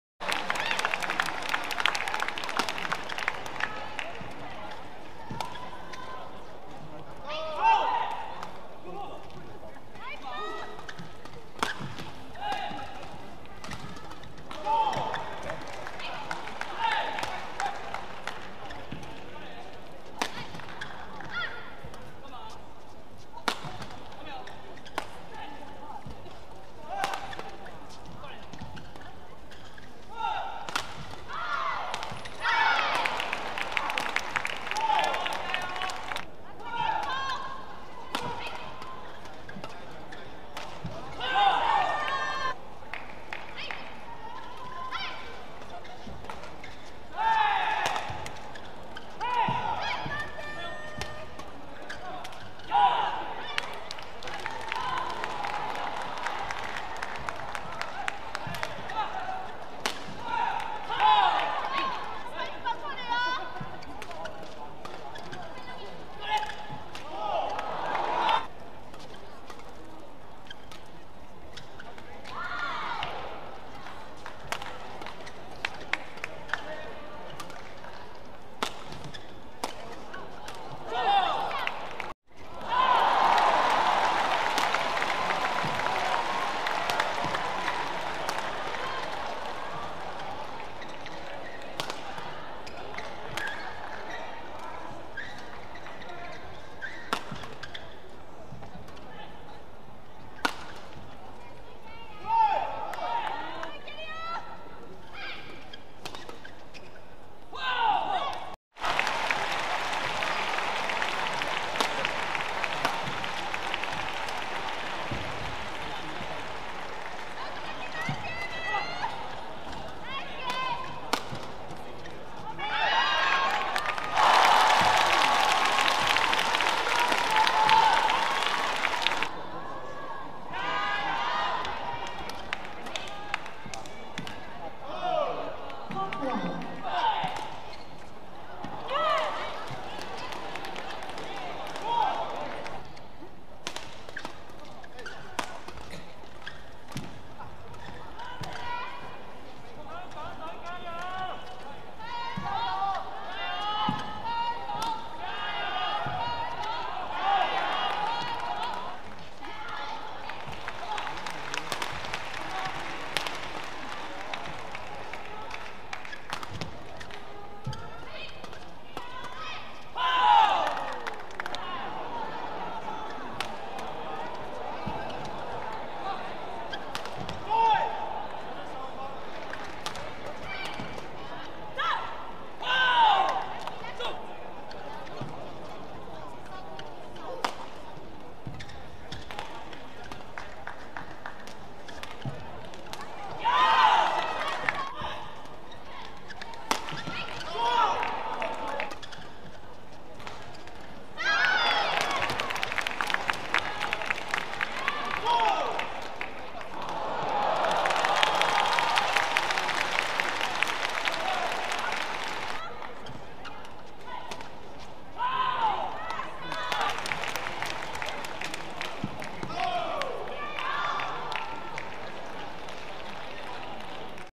VICTOR HONGKONG OPEN 2023 Round of 32 || Yeremia/Pramudya (INA) vs Ayato Endo/Yuta Takei (JPN)
This channel is only for sharing information about Badminton, the latest updates on world badminton tournaments.